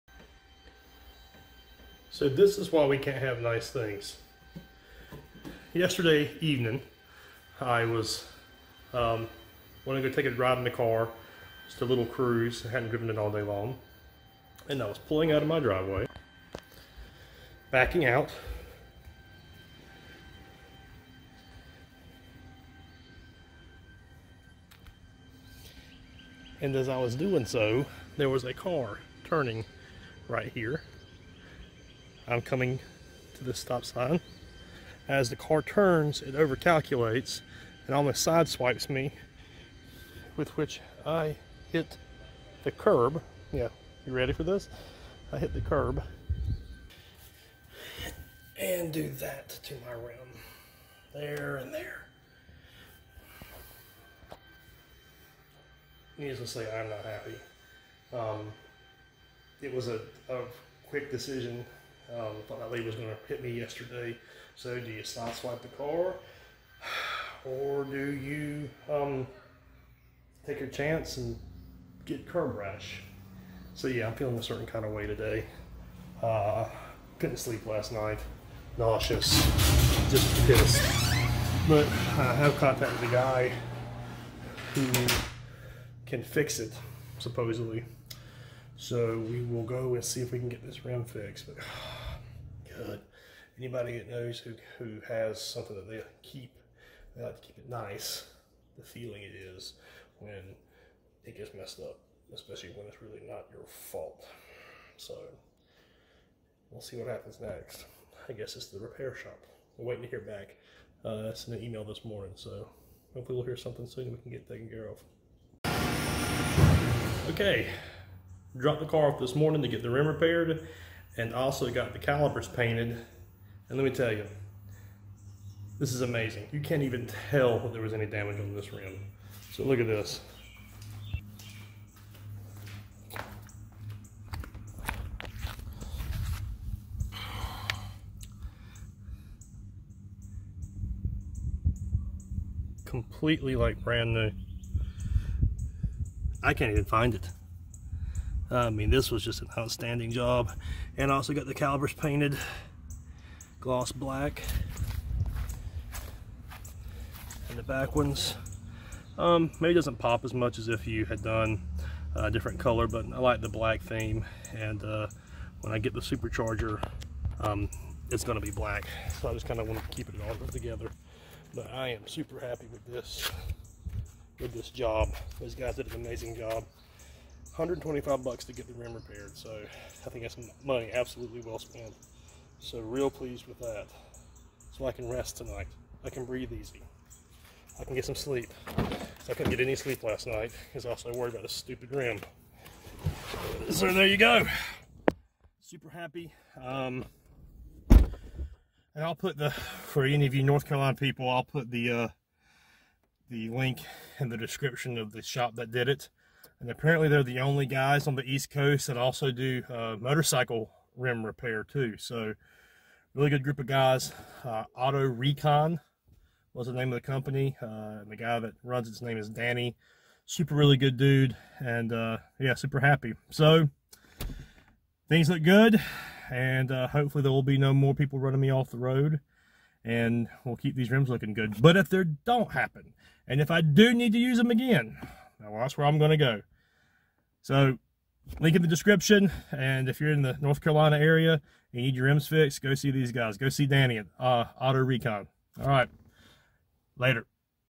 Ran off the road and damaged my NEW rims. This is why we can't have nice things.
Rim damage and subsequent repair for my GT Mustang.

Go see Danny for rim repair.  Tell him David sent you!